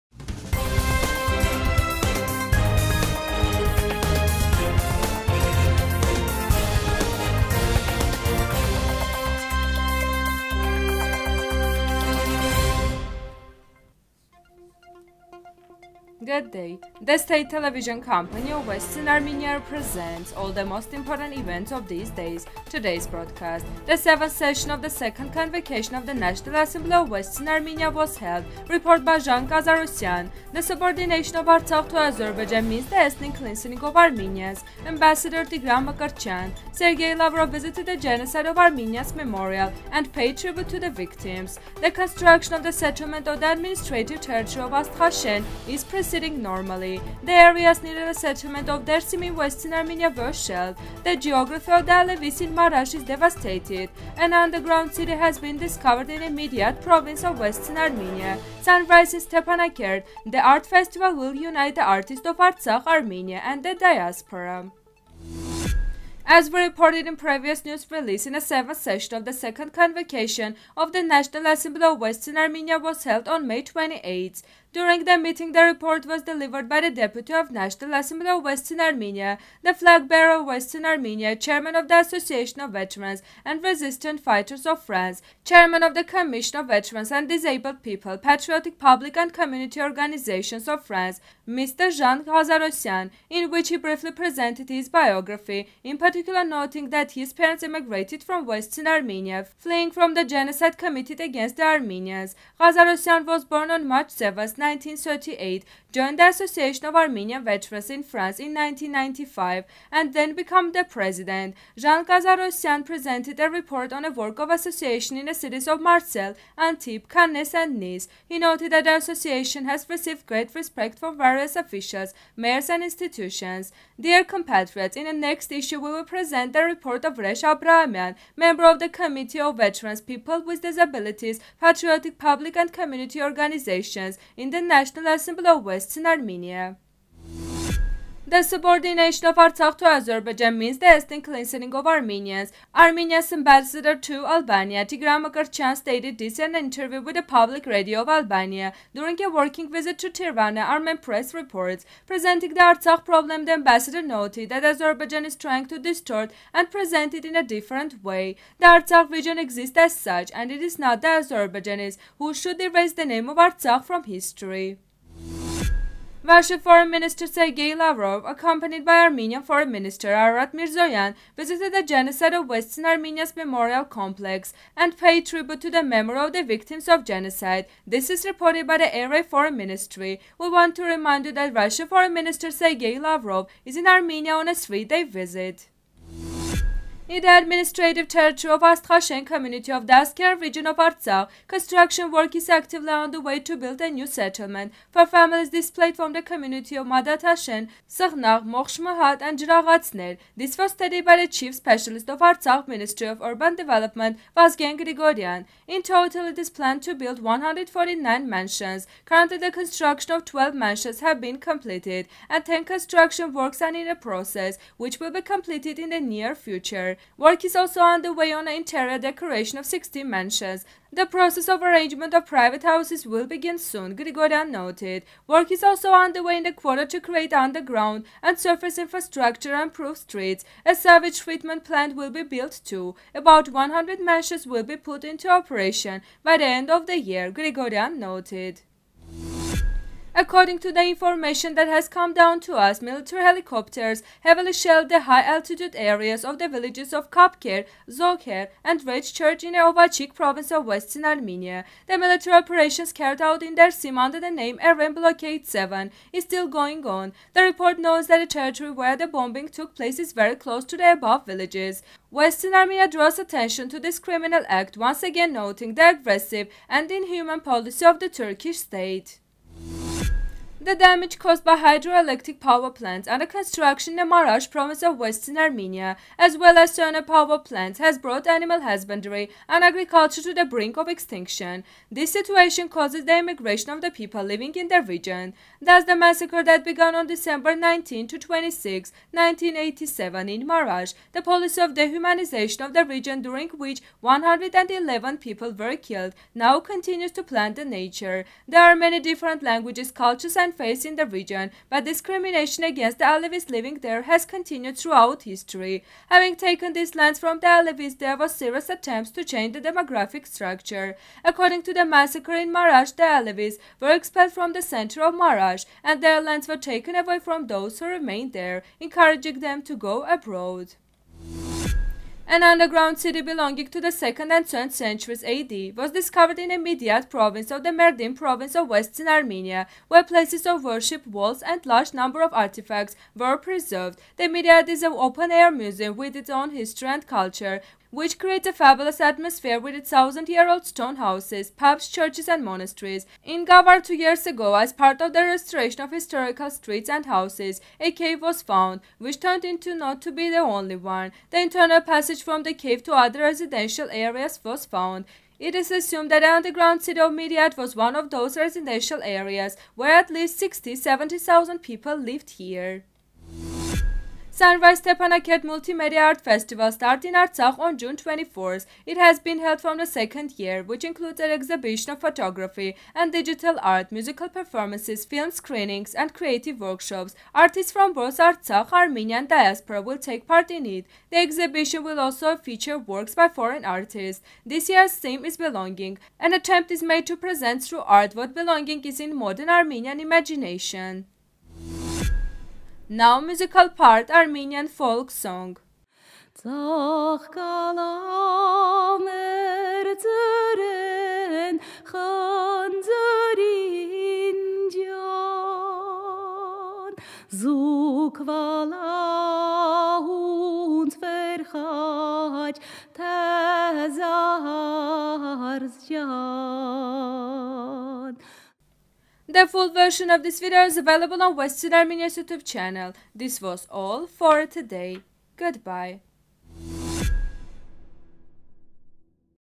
The 7th session of the 2nd convocation of the National Assembly of WA: report by Zhan Ghazarosian
1. The 7-th session of the 2nd convocation of the National Assembly of Western Armenia: report by Zhan Ghazarosian.
2. The subordination of Artsakh to Azerbaijan means the ethnic cleansing of Armenians. Ambassador Tigran Mkrtchyan.
3. Sergey Lavrov visited the  Genocide of Armenians Memorial and paid tribute to the victims.
4. The construction of the settlement on the administrative territory of Astkhashen is proceeding normally.
5. The areas near the settlements of Dersim in Western Armenia were shelled.
6. The geography of the Alevis in Marash is devastated.
7. An underground city has been discovered in the Midyat province of Western Armenia.
8. Sunrise in Stepanakert: the art festival will unite the artists of Artsakh, Armenia, and the Diaspora.